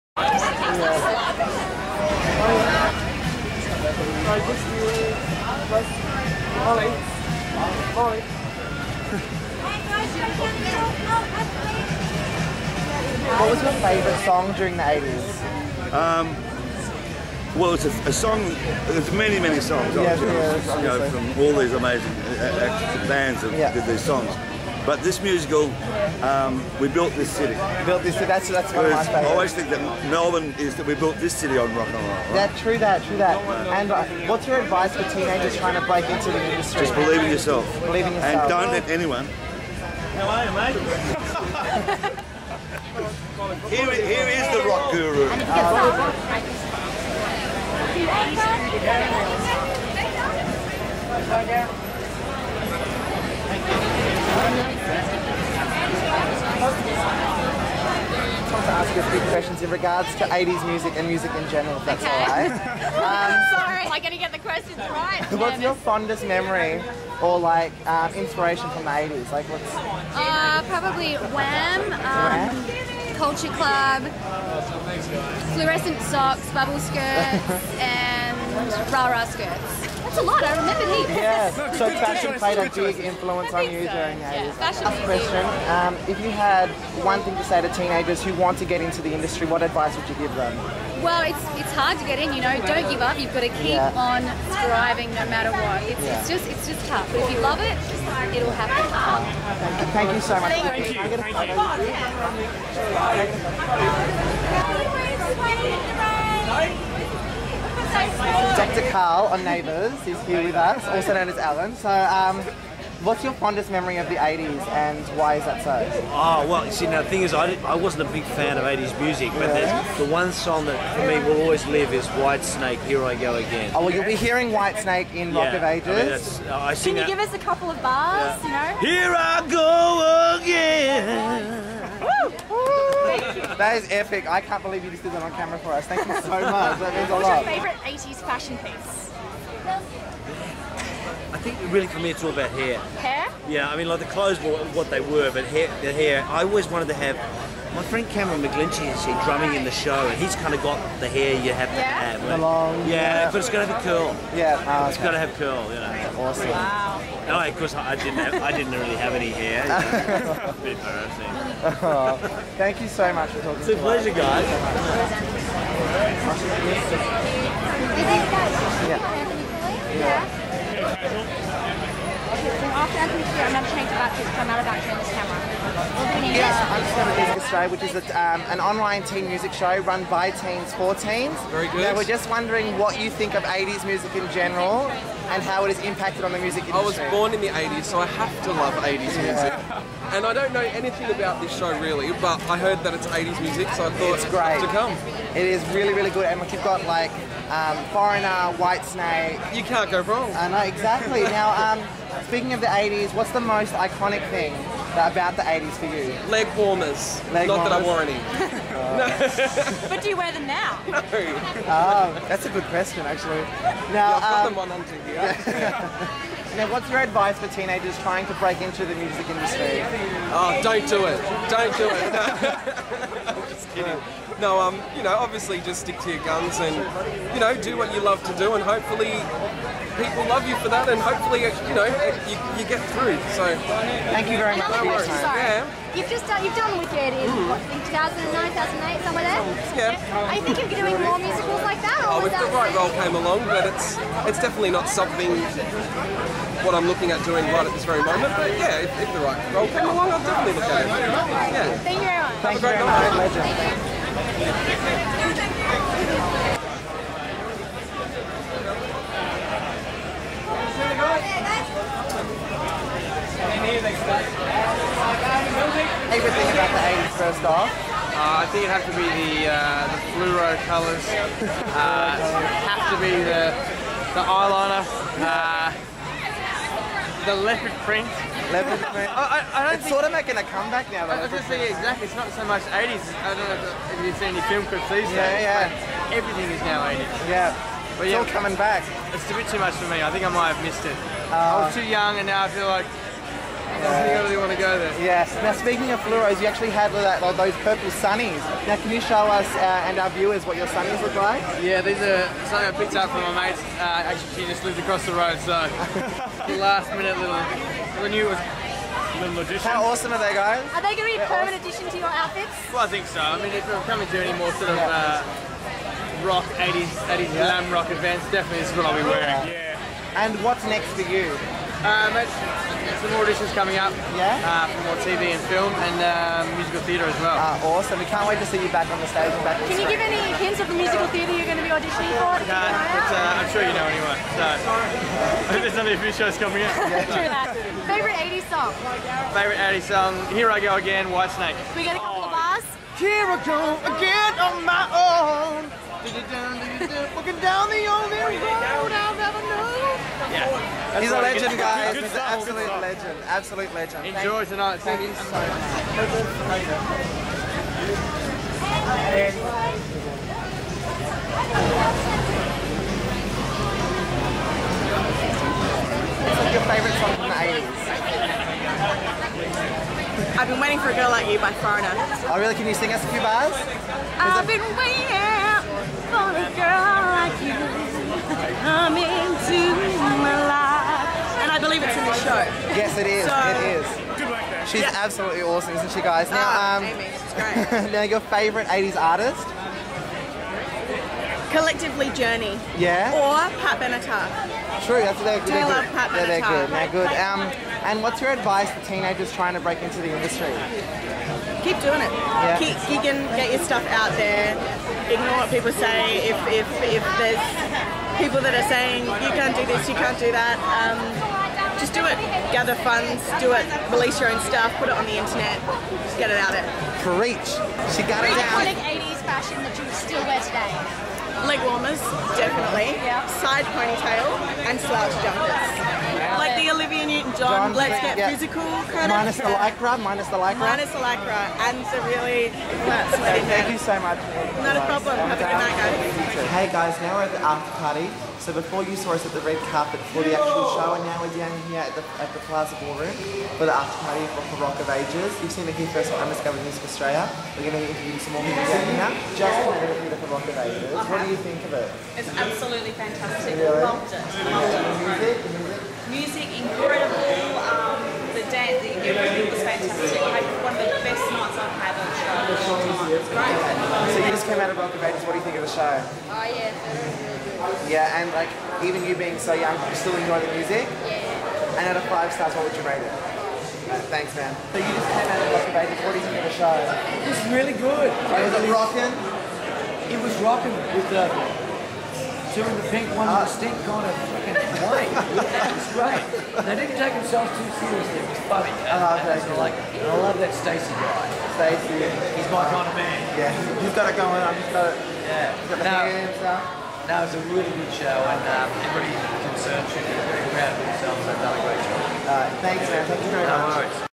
Rock of Ages Premier
The team from Undiscovered Music Australia interview the stars on the red carpet at the Austalian Premier of Australia's newest hit, feel good musical, Rock of Ages. Natalie Bassingthwaighte, Molly Meldrum and Anthony Callea (amongst others) talk about the 80's, the music and the life.